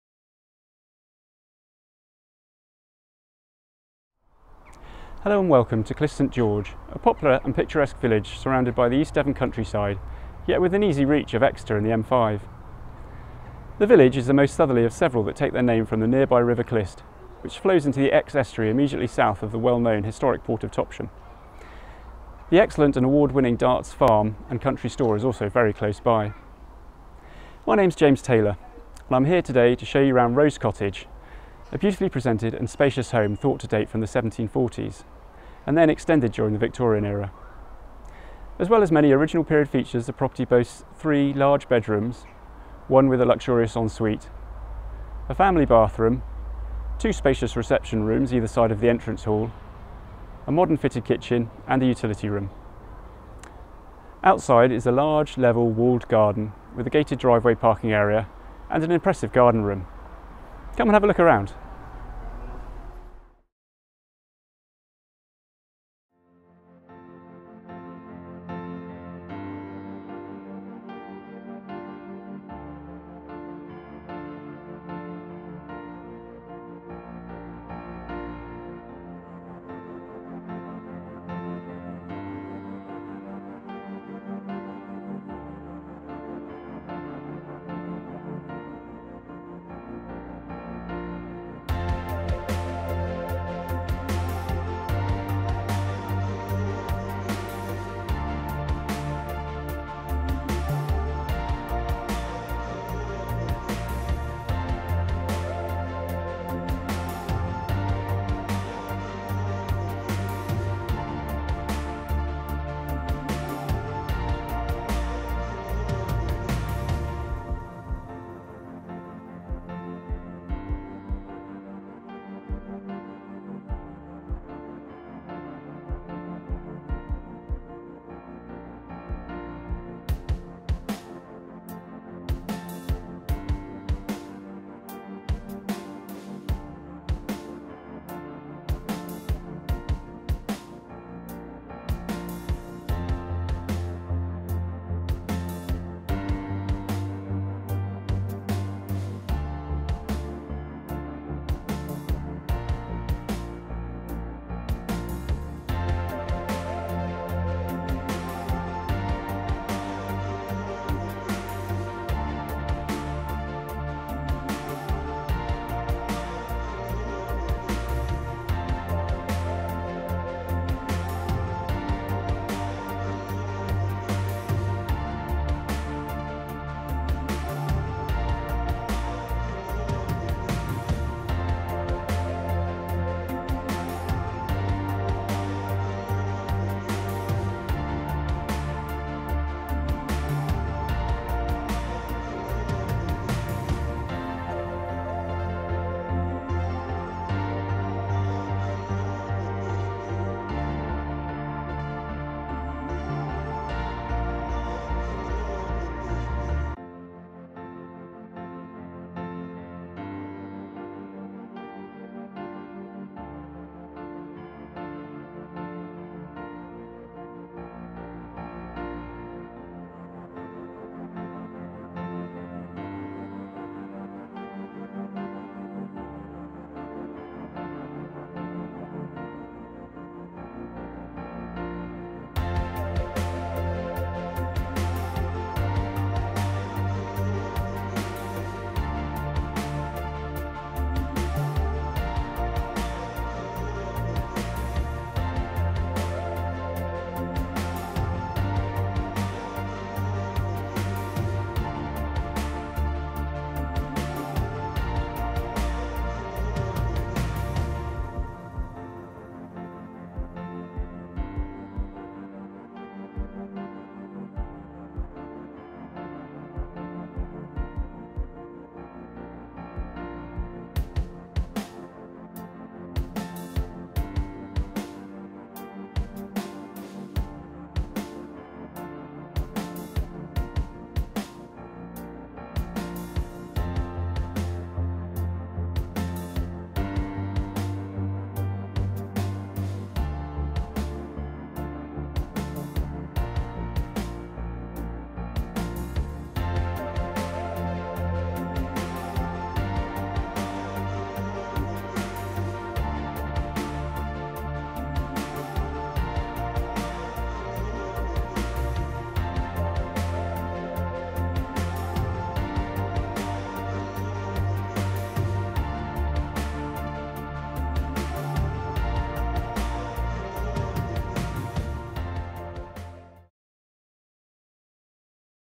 SOLD A beautiful East Devon Cottage close to Exeter and Topsham
A video tour of Rose Cottage in Clyst St George near Exeter and Topsham in East Devon.  This stunning cottage has ver 2,000sqft of space including 3 bedrooms (1 en suite) bathroom, 2 reception rooms, hallway, kitchen, utility room and cloakroom.  Out side is a large and private landscaped garden and gated driveway parking for 3 cars, as well as a large garden room.